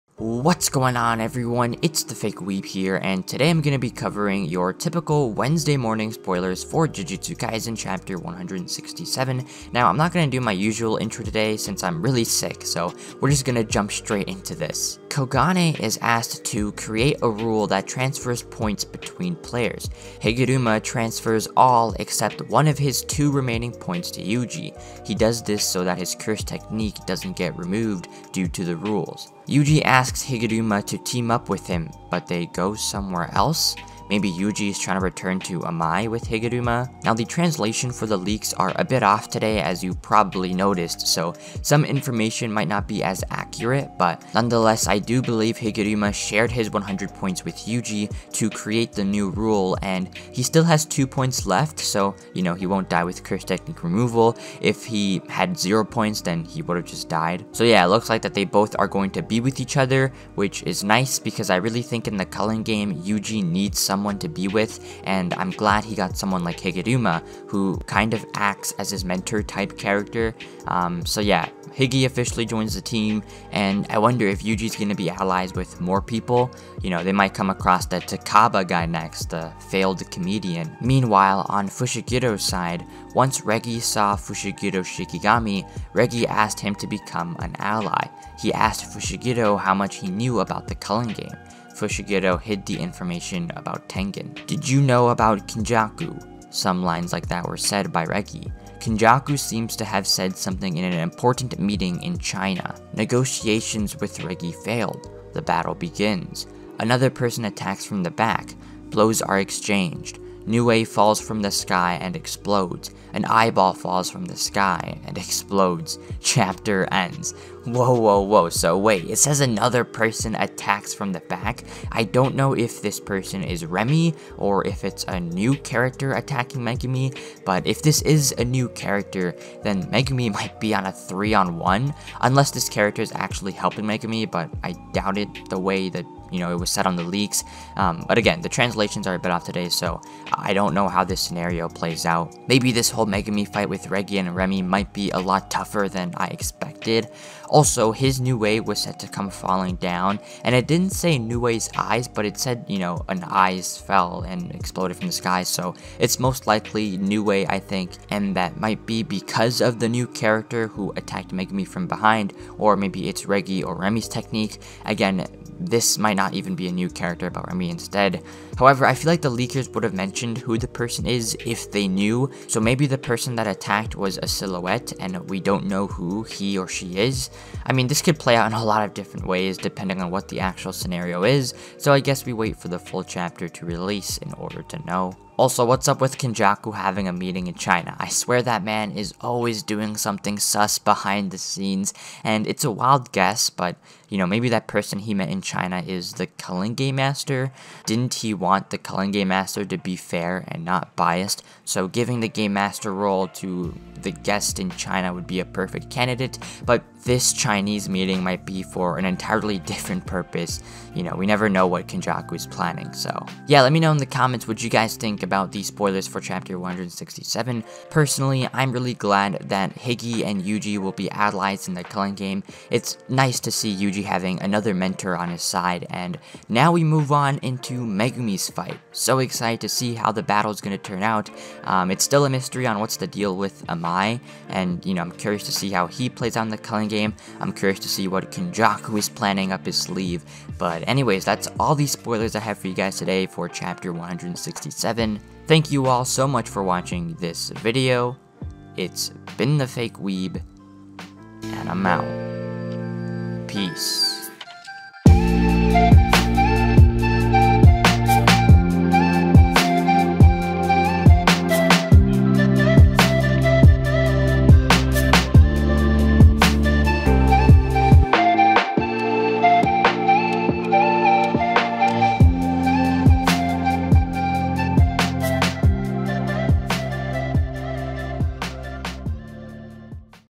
Jujutsu Kaisen Chapter 167 SPOILERS/LEAKS (JJK Manga)
Today I will be going over the spoilers that came out today for chapter 167 | The fight with Megumi starts now! also Kenjaku is sussy again...